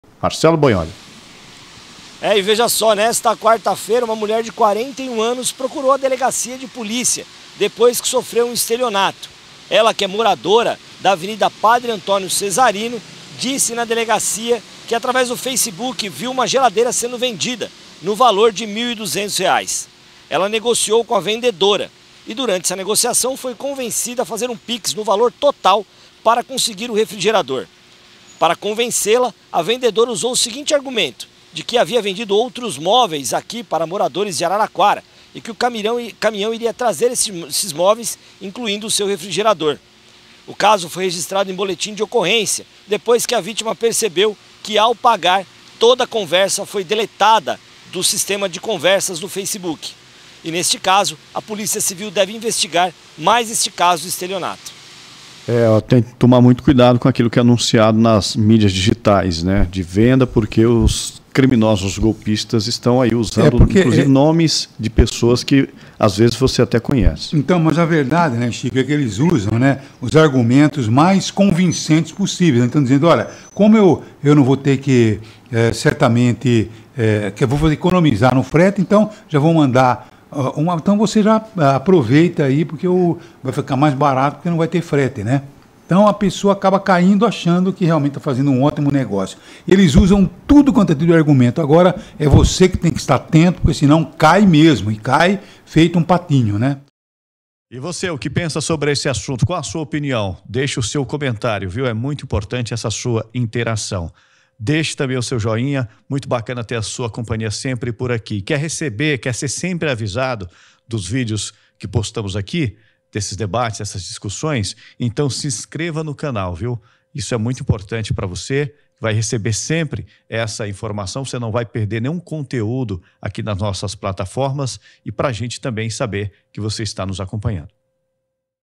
Mulher cai em golpe ao comprar geladeira pelo Facebook
Vítima pagou através de PIX antecipadamente e não obteve mais respostas da suposta vendedora.

Acompanhe o Portal Morada e fique por dentro das principais notícias do dia. As informações de Araraquara e região;